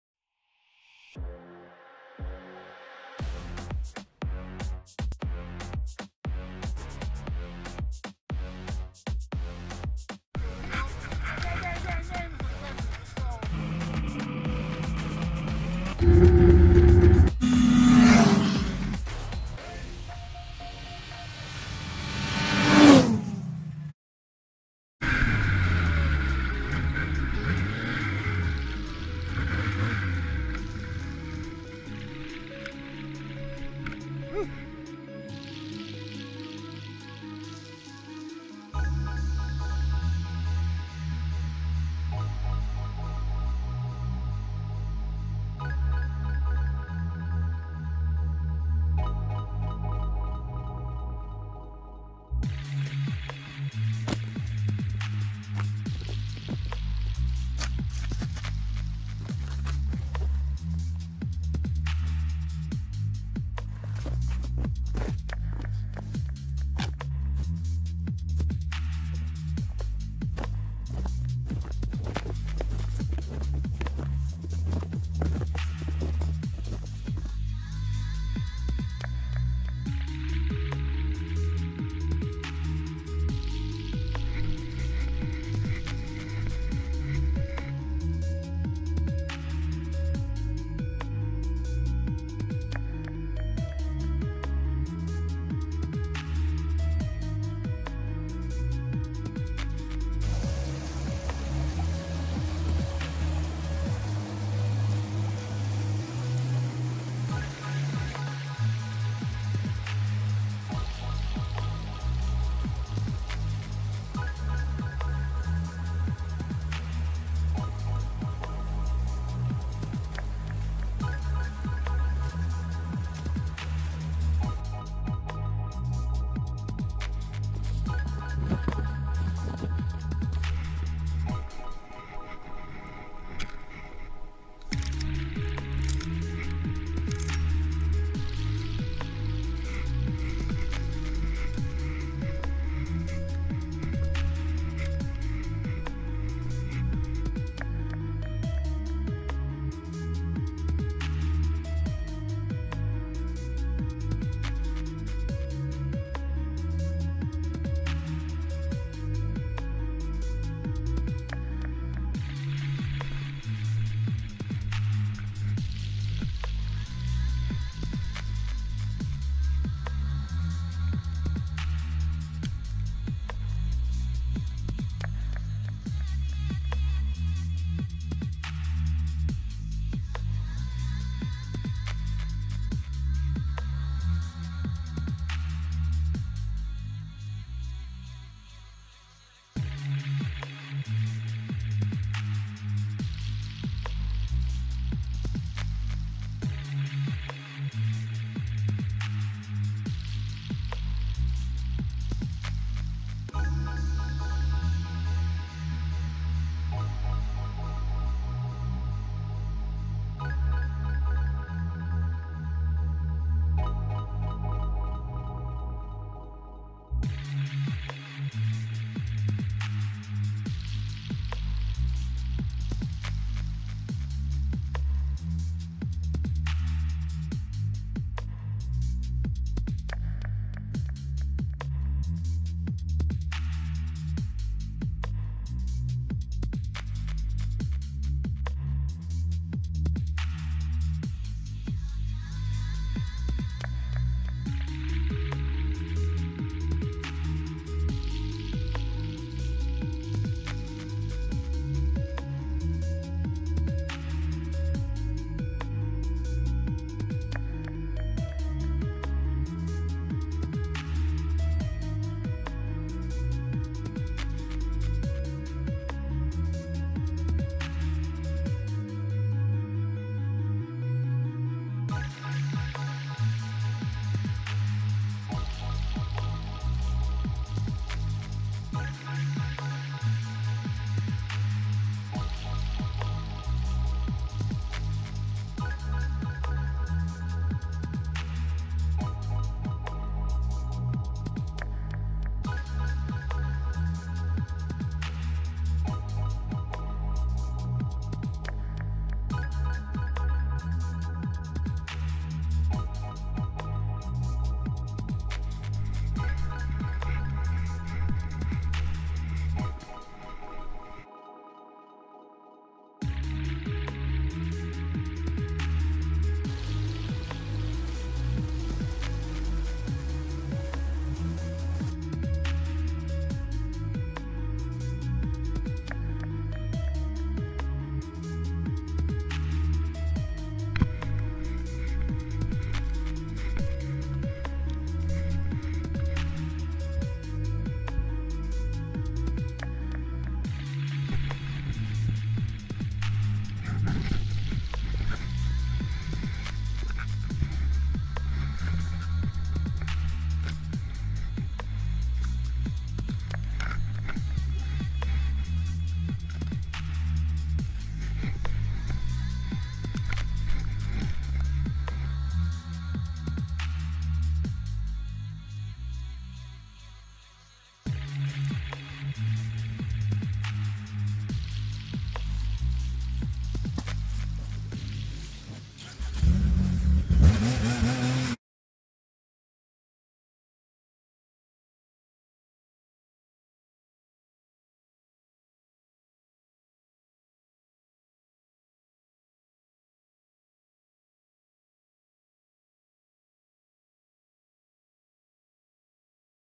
Blizzard Ride. Chapter 3 "The Explorer". Snowshoe to the Overlook. Incredible Views. Part 3 of 4
Having reached the trailhead, "The Explorer" straps on his snowshoes and journey to the overlook to catch some breathtaking vistas. The Moose Mountain Overlook peers directly down on the small town of Greenville in the Moosehead Lake Region of Maine. The views from this spot are absolutely astonishing, but don't take my word for it, see it here for yourself.